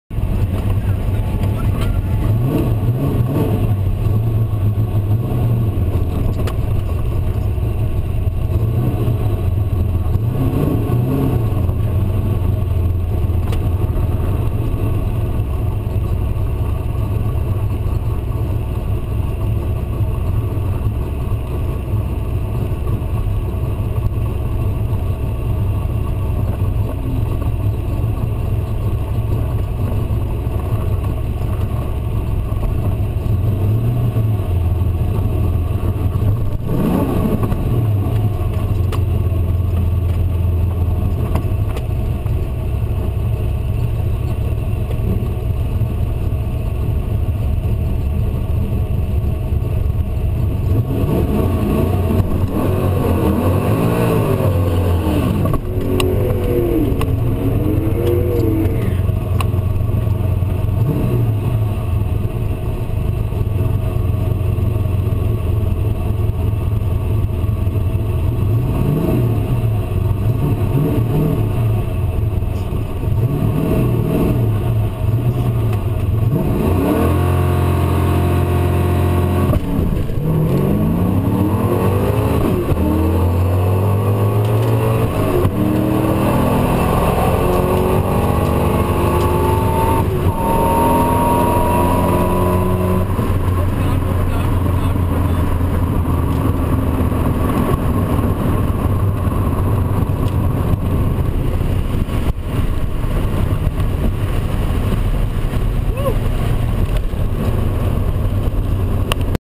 86 Capri 5.0 at ATCO - 1st Run GoPro - 12.423@113.44
A day at ATCO in my 86 ascMcLaren Capri 5.0.  This was an all motor pass - I did end up going 11.2 later that day with 150 shot.  I still can't get the launch down and my shifting still stinks but 10's are right around the corner.

Same run from outside the car.